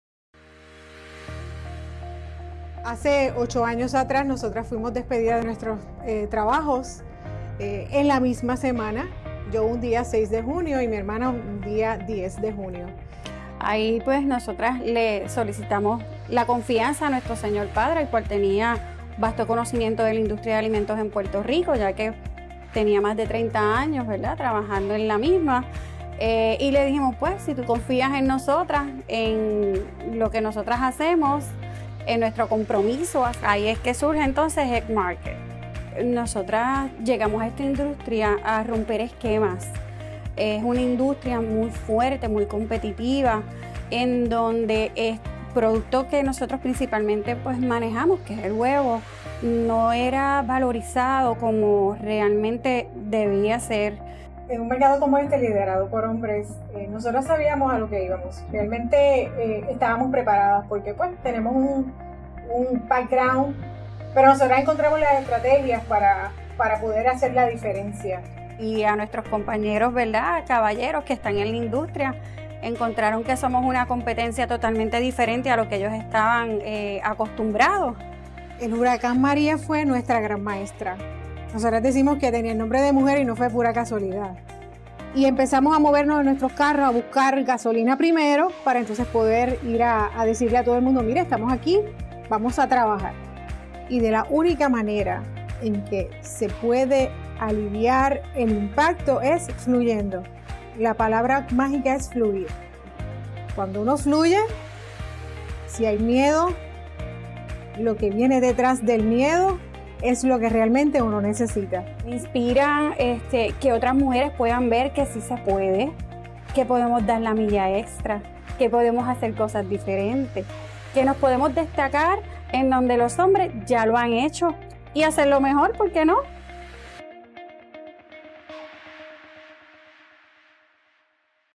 Historia de Éxito: Egg Market
Conoce cómo Ivette y Nydia Luciano superaron las adversidades y se reinventaron para lograr el éxito empresarial.